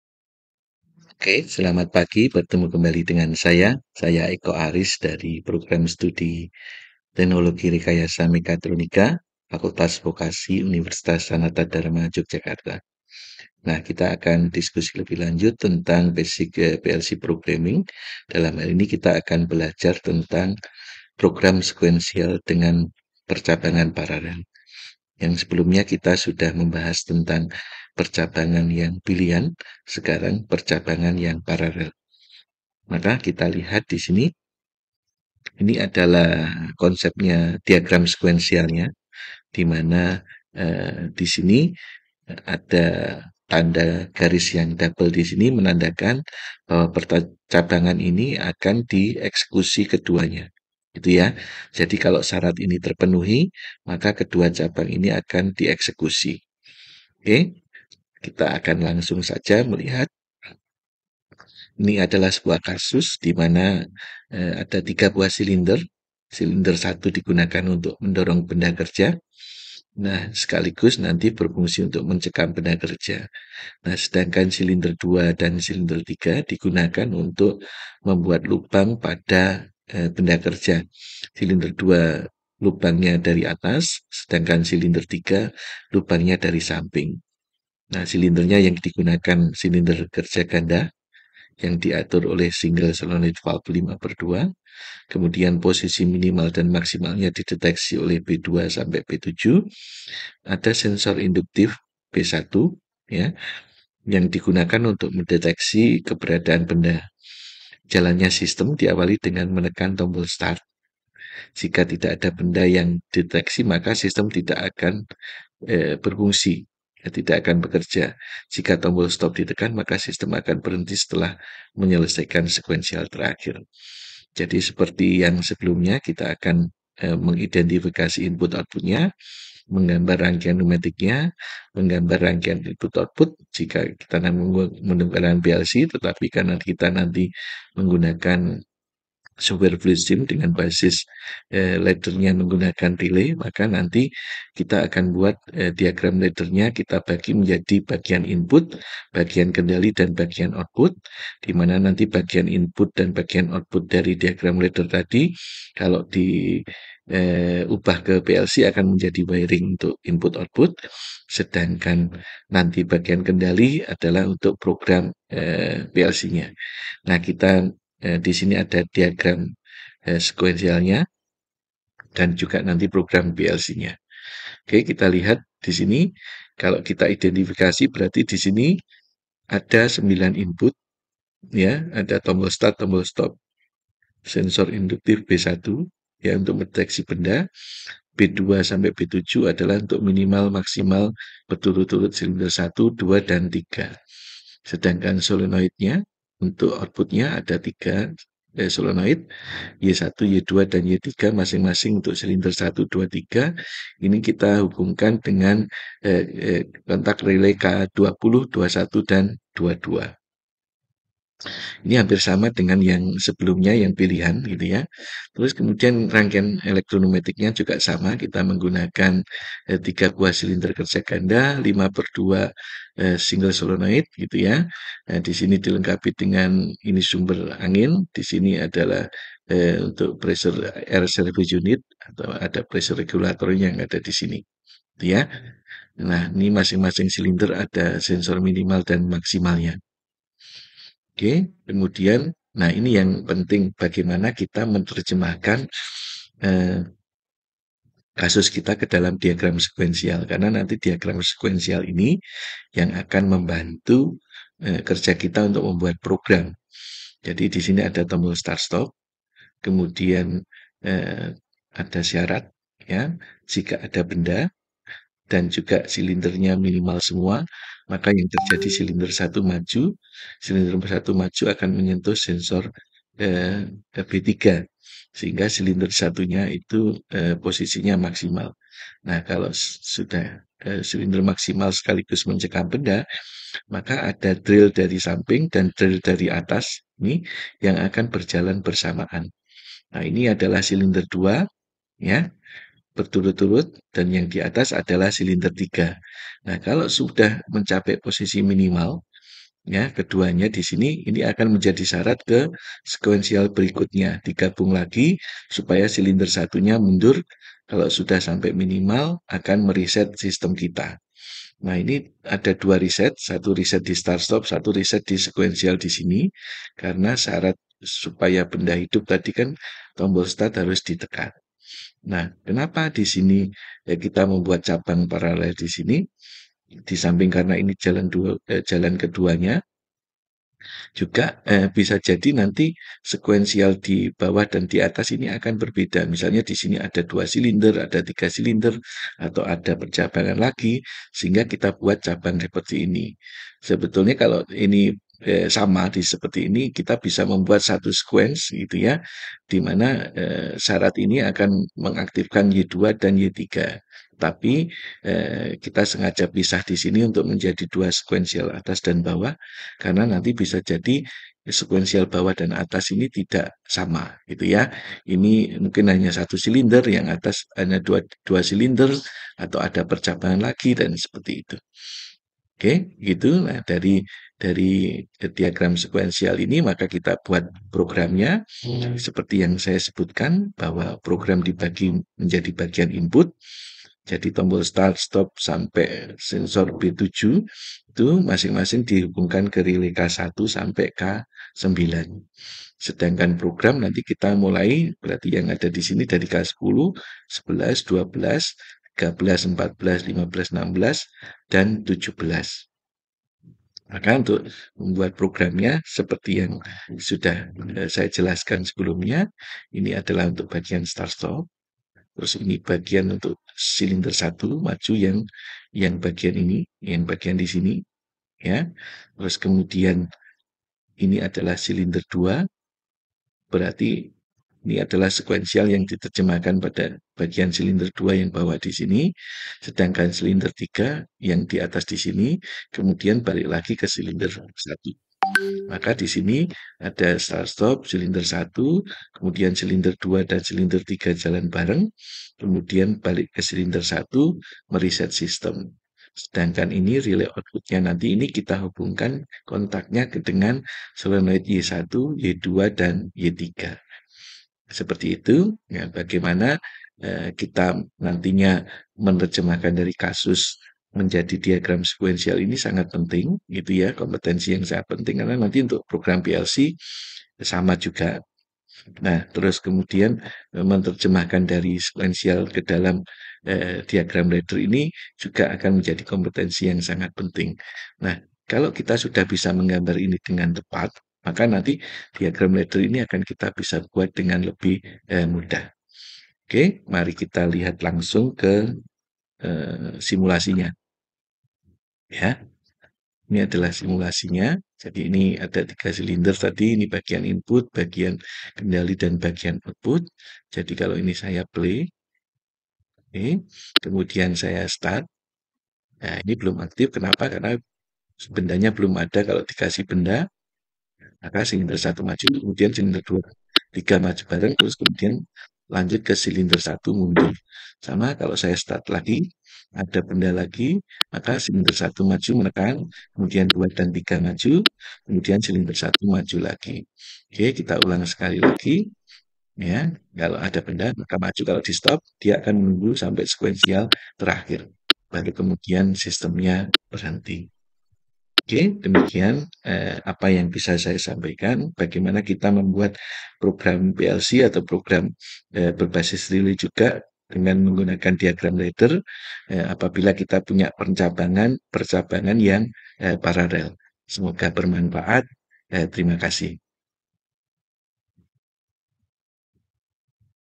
Tidak Punya Uang Untuk Kuliah // Dasar Pemrograman PLC Percabangan Parallel
Setelah sebelumnya dibahas Dasar Pemrograman PLC untuk Percabangan Pilihan, pada video kali ini akan dibahas tentang Dasar Pemrograman PLC untuk Percabangan Parallel. Simulasi menggunakan software Fluidsim Pneumatik berbasis Relay. Program dibuat sedemikian rupa supaya mempermudah saat akan dikonversi ke program berbasis PLC. Semoga bermanfaat